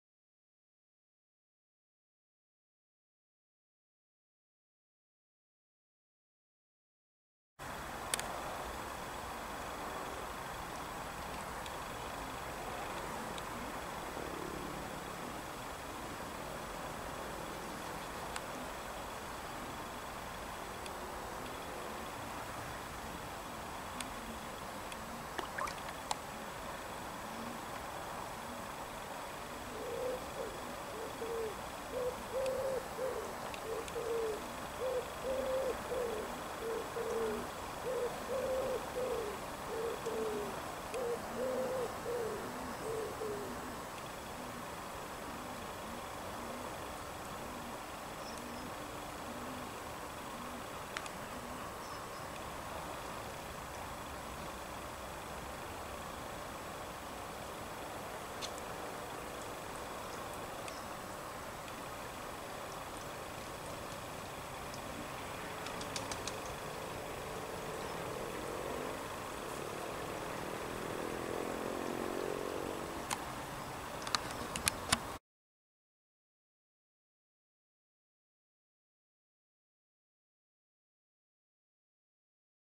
Large Pond Skater (Gerris najas)
Of all these years I've had pond skaters in my many ponds it the first time a large pond skater has landed in one! They are much larger that your common garden skater (Gerris lacustris).   Being larger they also tend to bully the smaller ones!  You never know what might land in your pond and that adds to the enjoyment of having a pond in your garden. It is not just what you put in, it is also what nature puts in!